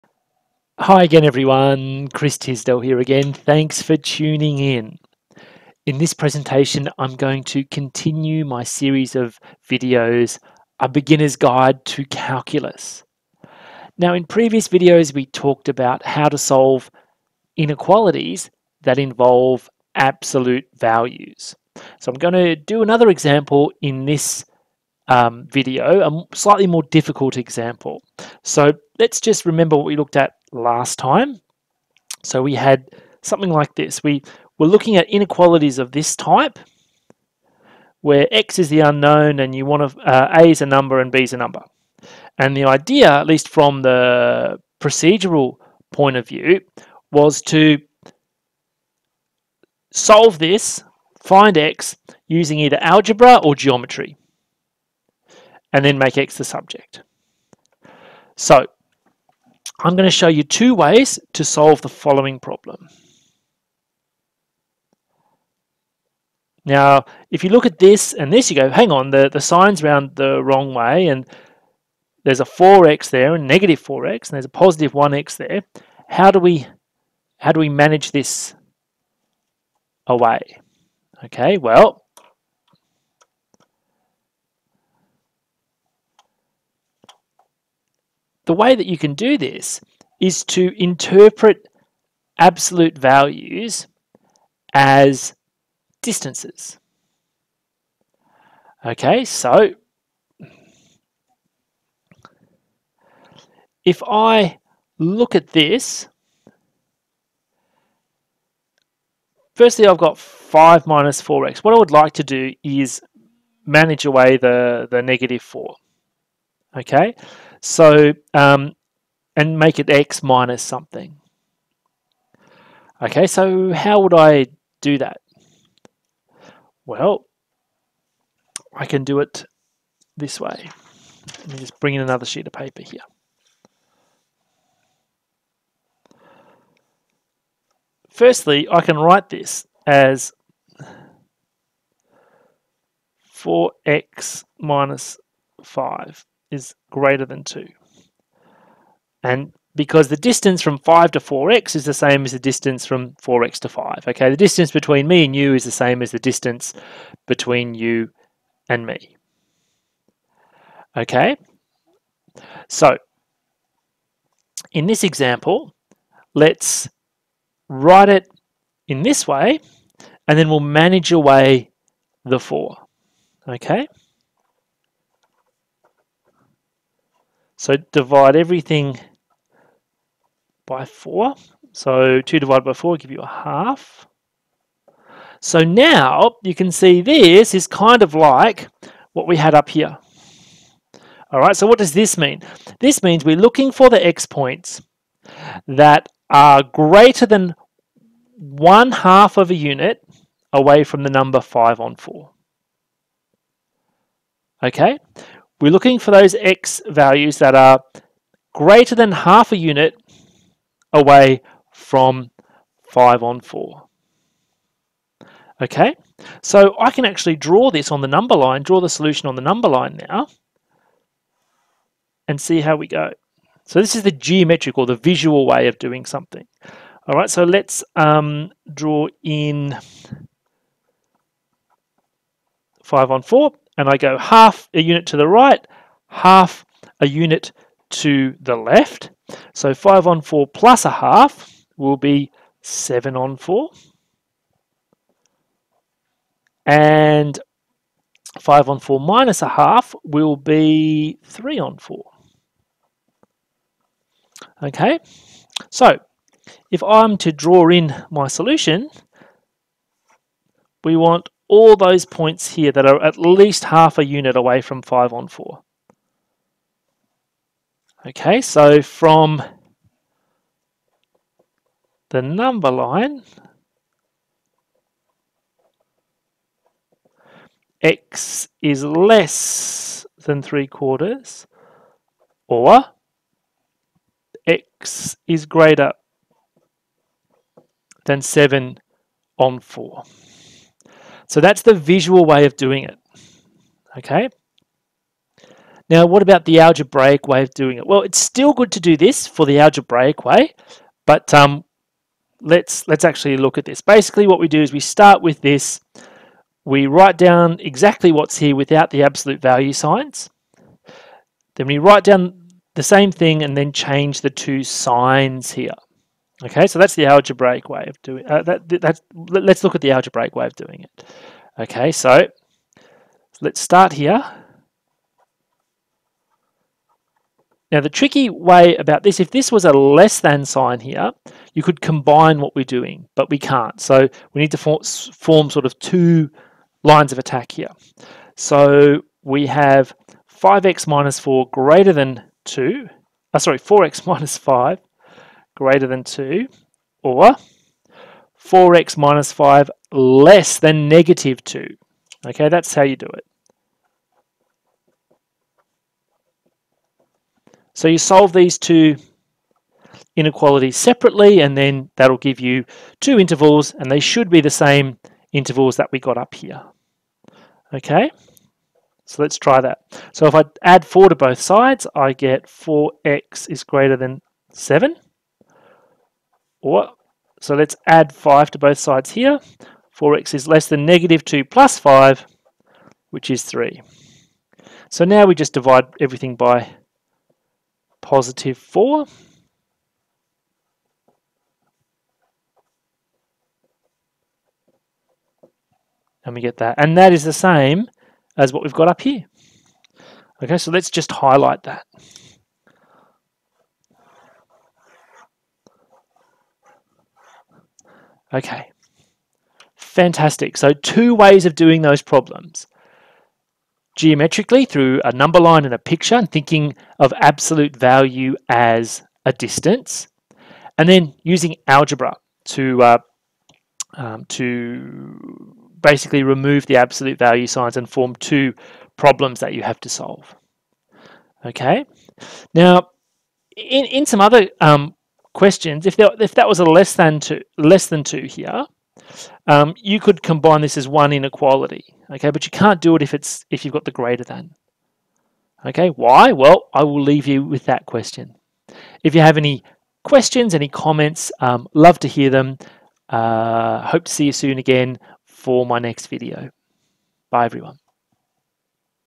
How to solve |5 - 4x| greater than 2: Dr Chris Tisdell Live Stream
This video shows how to solve an inequality involving absolute values.  I show two ways to solve the problem: using algebra; and using geometry.  Such ideas are important for solving all sorts of problems in mathematics.